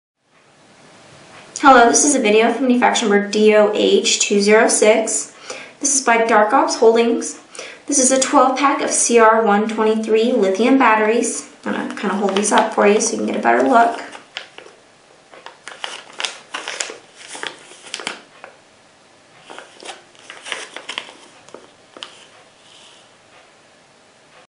Dark Ops Holdings 12 Pack of CR123 Lithium Batteries DOH206 Unboxing
This is our unboxing video for the 12 Pack of CR123 Lithium Batteries mfg# DOH206. At Wholesale Hunter